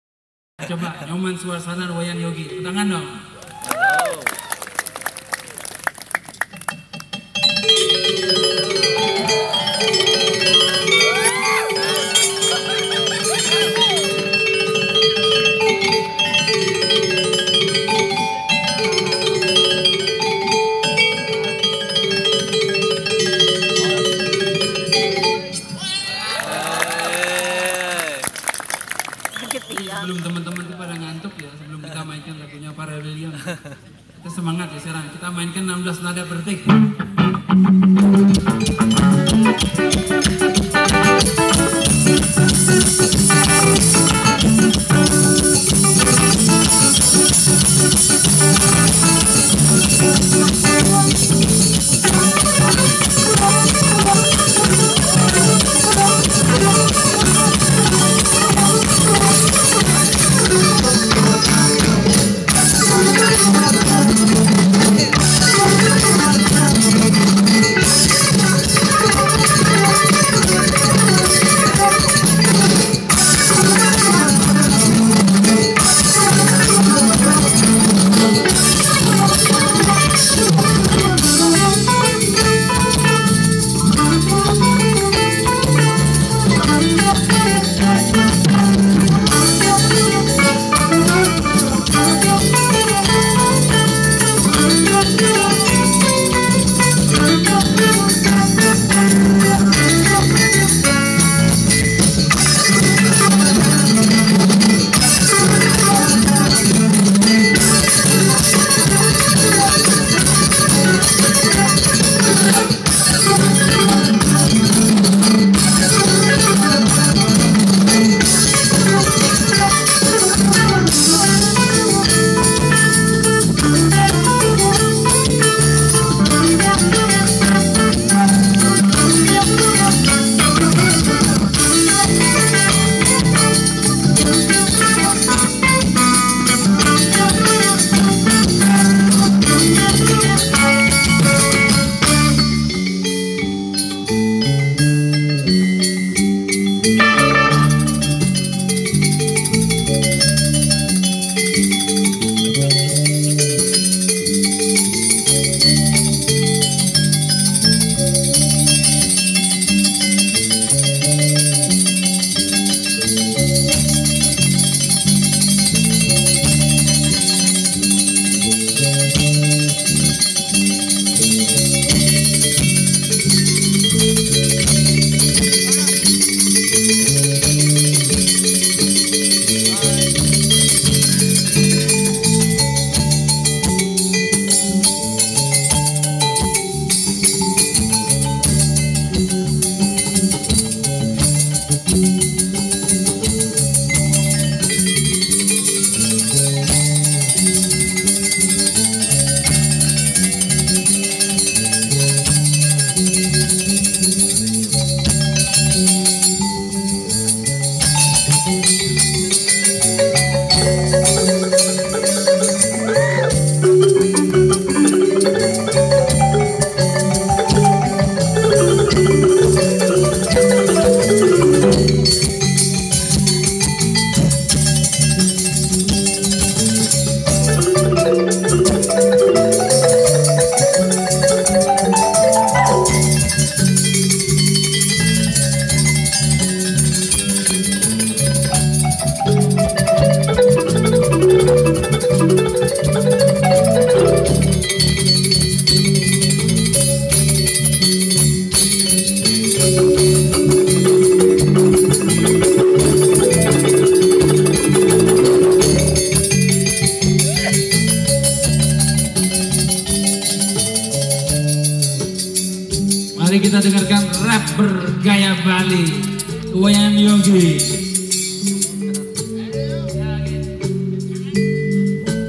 Musik Alam Fest 2K19 Wonderful Indonesia, sabtu 28 september 2019 (Taman Enggang, Tanjung Selor)
Musik Alam Fest 2K19 Wonderful Indonesia, sabtu 28 september 2019 (Taman Enggang, Tanjung Selor, Bulungan)

🎸 Balawan, 16 (enam belas nada) per detik 🎸